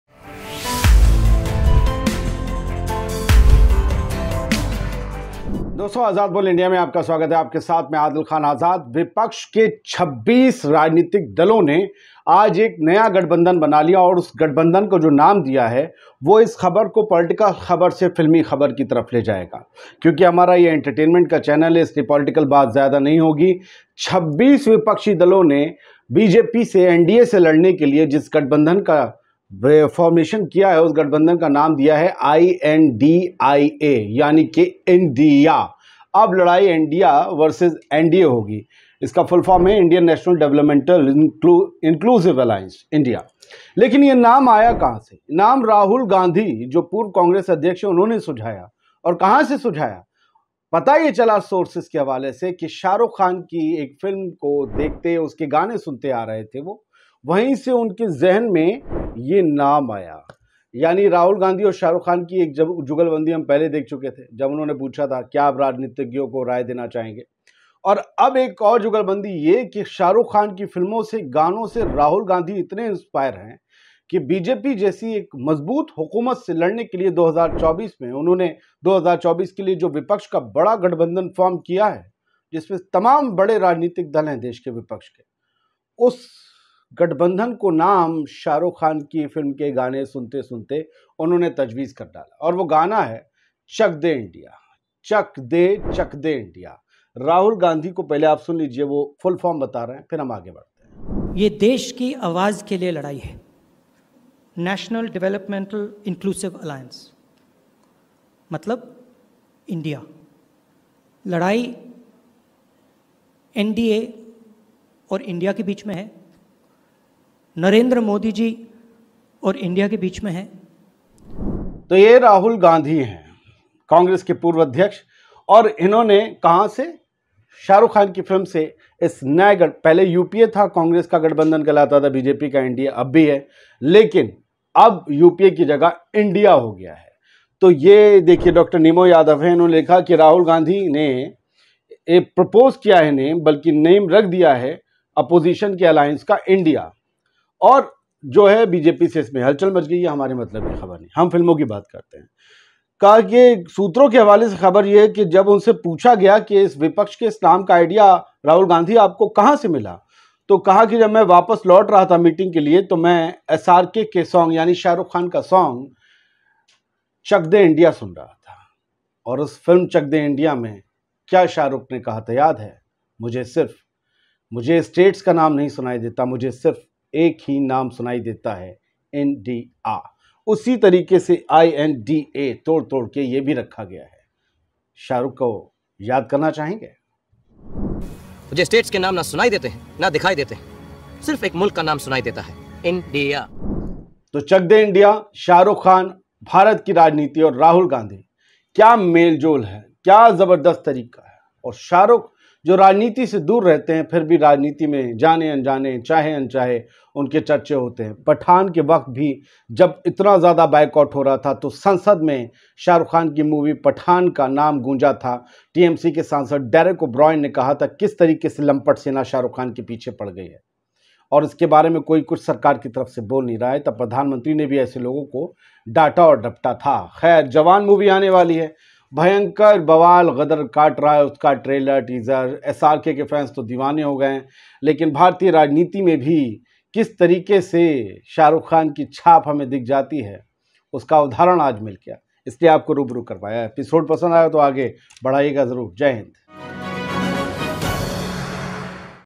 Shahrukh Khan के मुरीद हुए Rahul Gandhi? | Opposition meeting में Srk की Chak de से बना I.N.D.I.A ?
Shahrukh Khan के मुरीद हुए Rahul Gandhi?
Opposition meeting में Srk की Chak de से बना I.N.D.I.A ?